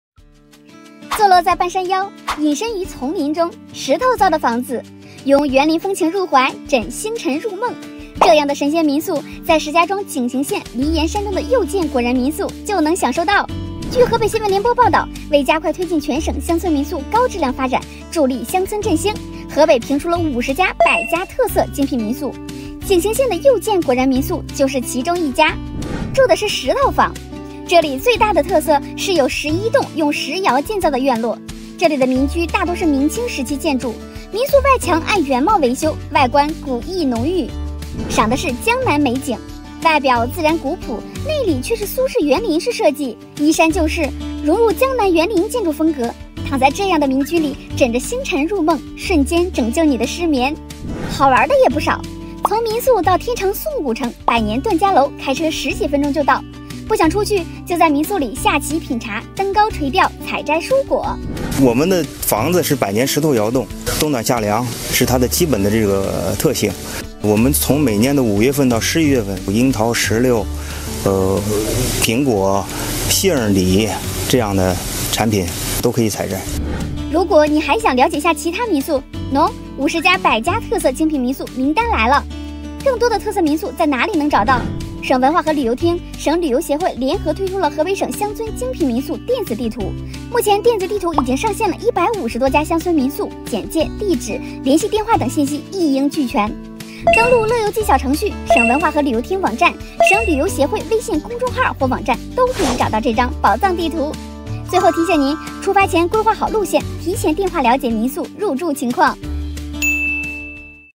“神仙民宿”啥体验？
坐落在半山腰，隐身于丛林中，石头造的房子，拥园林风情入怀，枕星辰入梦，这样的“神仙民宿”，在石家庄井陉县梨岩山庄的右见果然民宿就能享受到。

据河北新闻联播报道，为加快推进全省乡村民宿高质量发展，助力乡村振兴，河北评出了50家“百佳特色精品民宿”，井陉县的右见果然民宿就是其中一家。

住的是石头房。这里最大的特色是有11栋用石窑建造的院落，这里的民居大多是明清时期建筑，民宿外墙按原貌维修，外观古意浓郁。

赏的是江南美景。外表自然古朴，内里却是苏式园林式设计，依山就势，融入江南园林建筑风格。躺在这样的民居里枕着星辰入梦，瞬间拯救你的失眠。

好玩儿的也不少。从民宿到天长宋古城、百年段家楼，开车十几分钟就到。不想出去，就在民宿里下棋品茶、登高垂钓、采摘蔬果。

如果你还想了解下其他民宿，喏，50家“百佳特色精品民宿”名单来了！

更多的特色民宿在哪里能找到？省文化和旅游厅、省旅游协会联合推出了《河北省乡村精品民宿电子地图》，目前电子地图已经上线了150多家乡村民宿，简介、地址、联系电话等信息一应俱全。

登录乐游冀小程序、省文化和旅游厅网站、省旅游协会微信公众号或网站，都可以找到这张宝藏地图！

最后提醒您，出发前规划好路线，提前电话了解民宿入住情况。